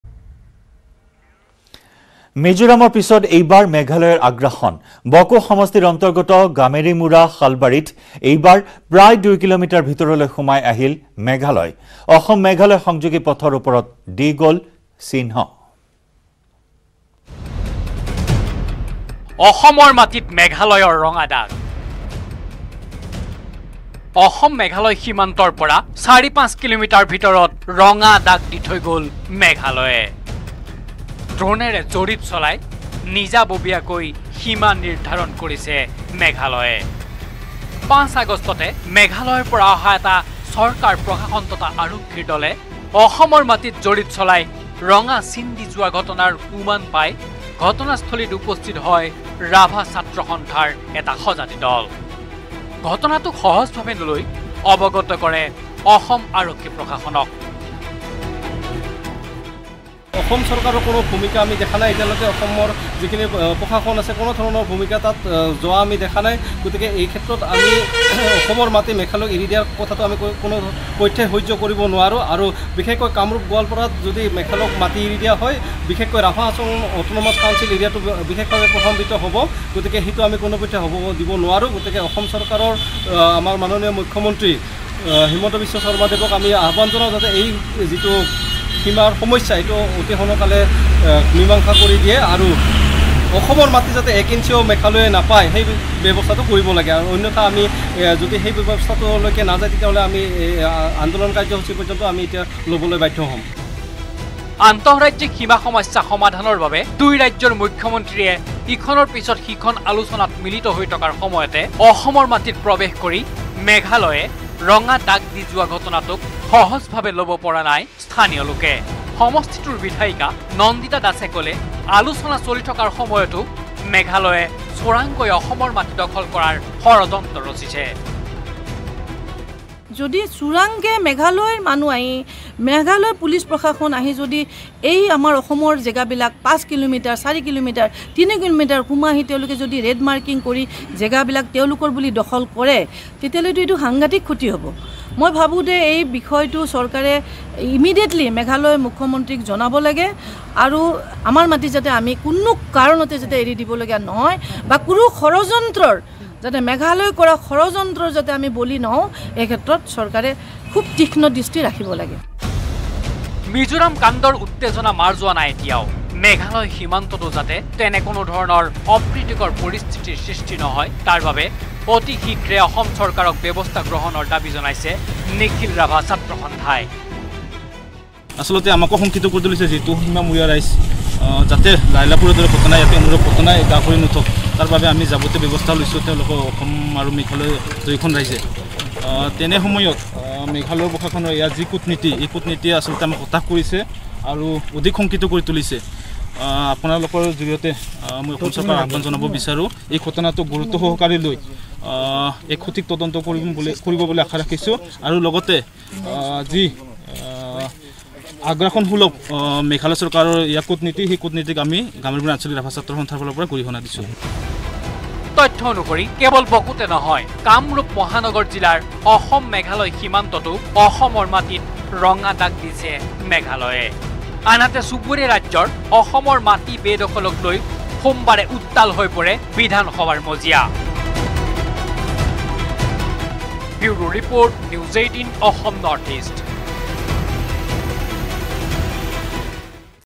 Meghalaya News | অসম-মেঘালয় সংযোগী পথত মেঘালয়ৰ দাগ
9 August, 2021: মিজোৰামৰ পিছত এইবাৰ মেঘালয়ৰ আগ্ৰাসন। বকো সমষ্টিৰ অন্তৰ্গত গামেৰীমুড়া শালবাৰীত আগ্ৰাসন। প্ৰায় ২ কিলোমিটাৰ ভিতৰলৈ সোমাই আহিল মেঘালয়।